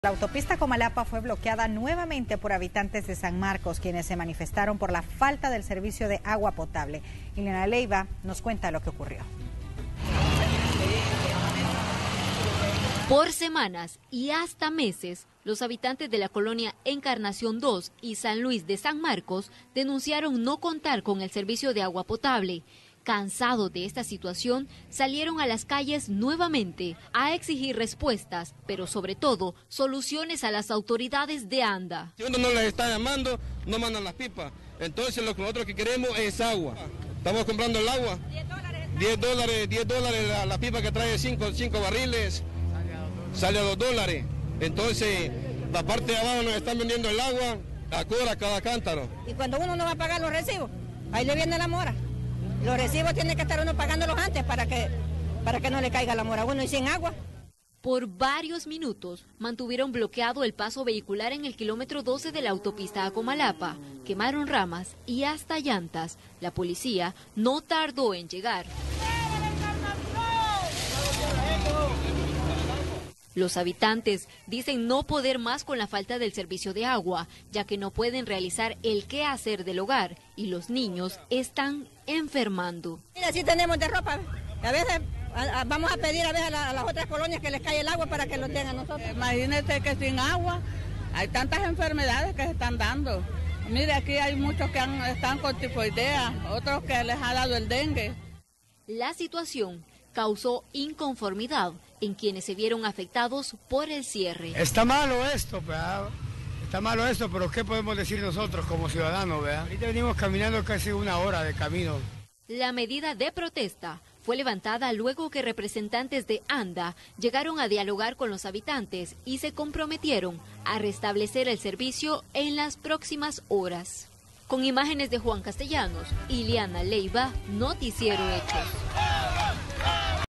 Bloquearon autopista a Comalapa para exigir agua potable
Habitantes de San Marcos no cuentan con el servicio por semanas o meses. Exigen soluciones a las autoridades de ANDA.